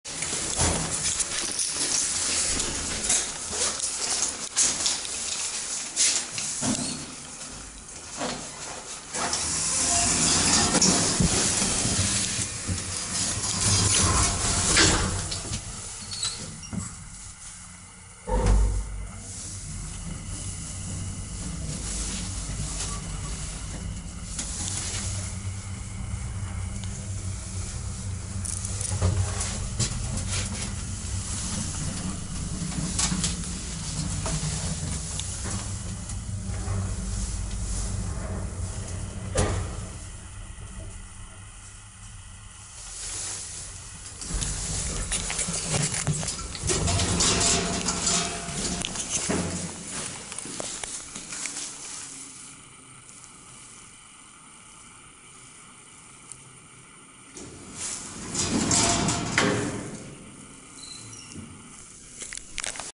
Электрический лифт (село Непецино), V=1 м-с, г-п 400 кг (2)
Тимохина 26
P.S. К сожалению без вида из окна, т.к. очень сильно торопился я и плюс вид был одинаков, что из 25 дома по той же улице